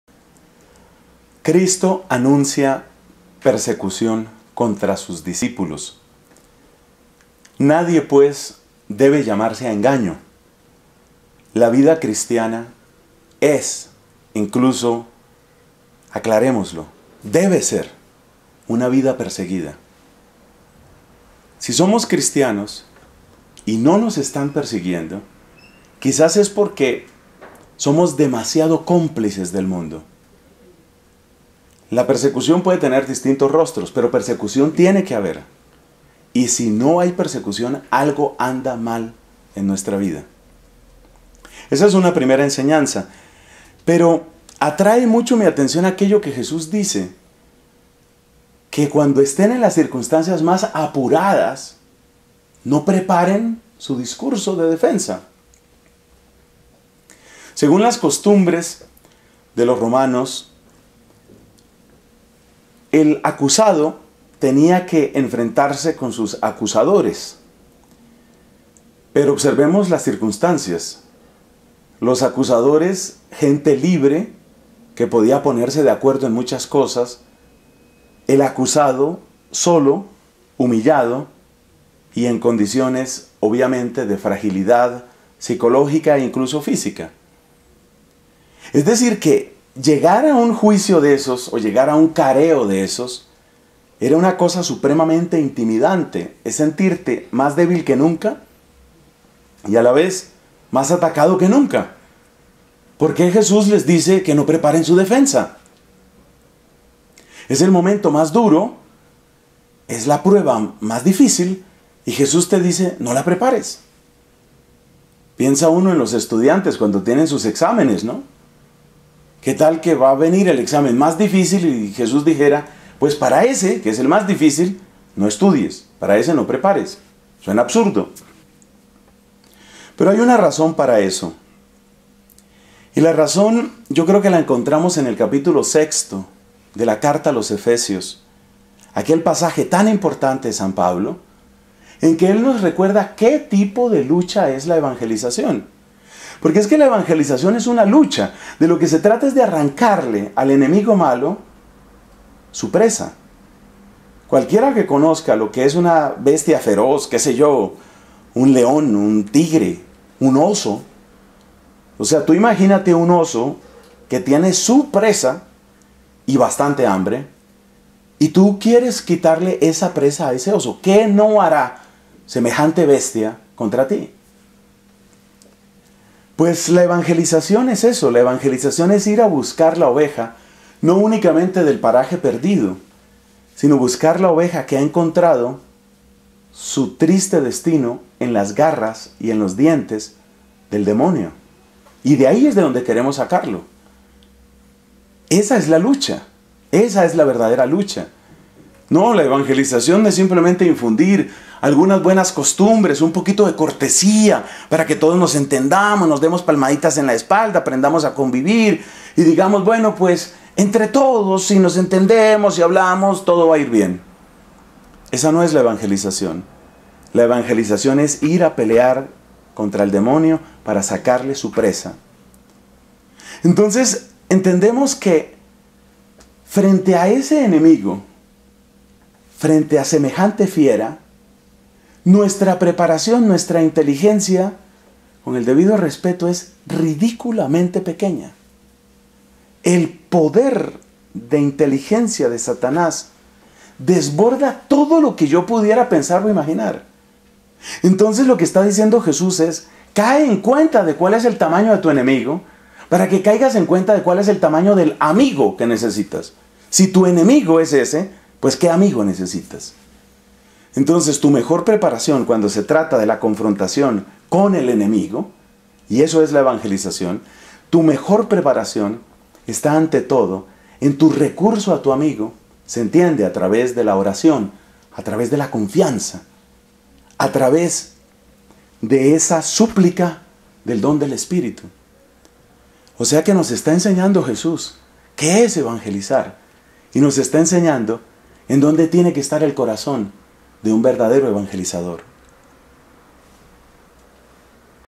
LA GRACIA del Miércoles 27 de Noviembre de 2013
Cristo advierte que no debe fiarse uno de las propias fuerzas porque la batalla es contra los espíritus del mal.